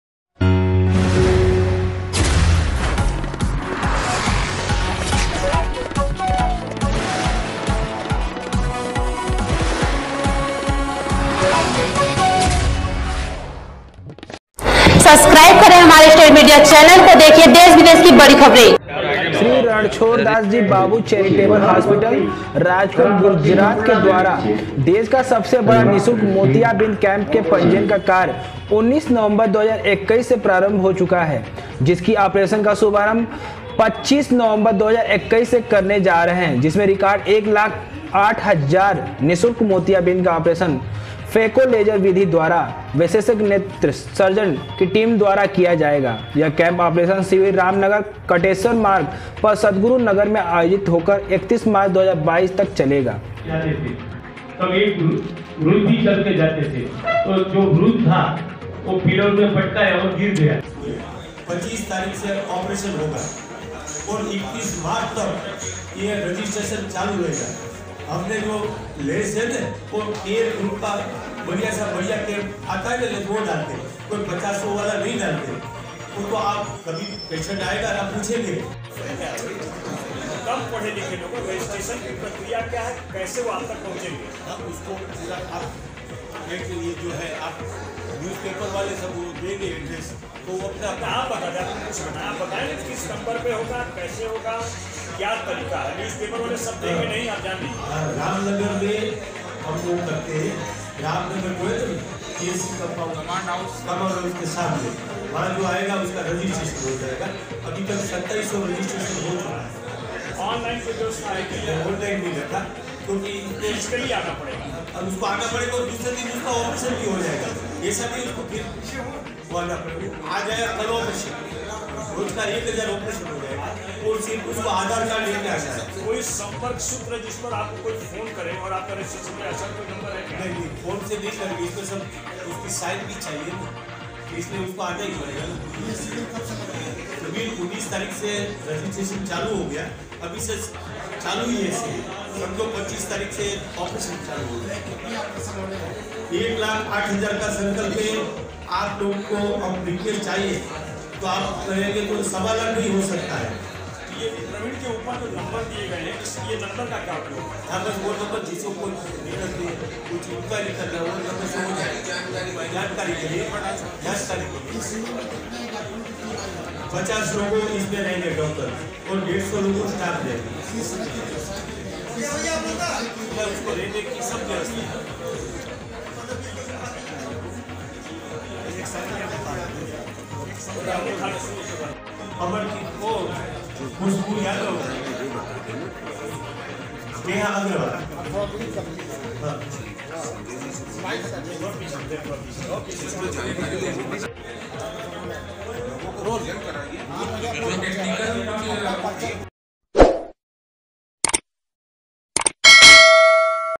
'श्री रणछोड़ दास जी बापू चैरिटेबल हॉस्पिटल' ने लगाया सबसे बड़ा निःशुल्क मोतियाबिंद शिविर.
वाराणसी के रामनगर में 'श्री रणछोड़ दास जी बापू चैरिटेबल हॉस्पिटल' ने लगाया सबसे बड़ा निःशुल्क मोतियाबिंद शिविर.

श्री रणछोड़ दास जी बापू चैरिटेबल हॉस्पिटल राजकोट गुजरात के द्वारा देश का सबसे बड़ा निशुल्क मोतियाबिंद कैंप के पंजीयन का कार्य 19 नवंबर 2021 से प्रारंभ हो चुका है, जिसकी ऑपरेशन का शुभारंभ हम 25 नवंबर 2021 से करने जा रहे हैं जिसमें रिकॉर्ड 1 लाख 8000 निशुल्क मोतियाबिंद का ऑपरेशन फेको लेजर विधि द्वारा विशेषज्ञ नेत्र सर्जन की टीम द्वारा किया जाएगा ।यह कैंप ऑपरेशन शिविर रामनगर कटेसर मार्ग पर सद्गुरु नगर में आयोजित होकर 31 मार्च 2022 तक चलेगा। प्रतिदिन लगभग 1000 ऑपरेशन कैंप में किए जाएंगे इसका रजिस्ट्रेशन 19 नवंबर सन  2021 से प्रारंभ हो गया है

इसी इसी कड़ी में आज भेलूपुर स्थित एक होटल में प्रेस वार्ता करते हुए संस्था के ट्रस्टी श्री प्रवीण भाई ने बताया कि
"श्री रणछोड़ दास जी बापू चैरिटेबल आई हॉस्पिटल राजकोट गुजरात ने पत्रकारों को जानकारी देते हुए बताया उन्होंने आगे बताया कि इस शिविर से जहां उत्तर प्रदेश बिहार झारखंड मध्य प्रदेश छत्तीसगढ़ एवं अन्य राज्यों के लोगों को लाभ मिलेगा यह काम पूरी तरह निशुल्क है यहां रहने के लिए प्रबंध है भोजन का प्रबंध  ट्रस्ट द्वारा की जा रही है"
सह  संयोजक एवं दिव्यांग प्रकोष्ठ भारतीय जनता पार्टी काशी महानगर के संयोजक डॉ संजय चौरसिया जी ने  प्रेस वार्ता को संबोधित करते हुए कहा कि सेवा ही सबसे बड़ा धर्म है और पूज्य रणछोड़ दास जी महाराज  के आशीर्वाद से जो काशी में शिविर लग रहा है वह अनुपम एवं अलौकिक है  इससे लाखों मरीज लाभान्वित होंगे, उन्होंने लोगों का आह्वान किया कि अधिक से अधिक लोग इस शिविर में आए और इस शिविर का लाभ लें काशी की धरती पर इस प्रकार का सबसे बड़ा आयोजन है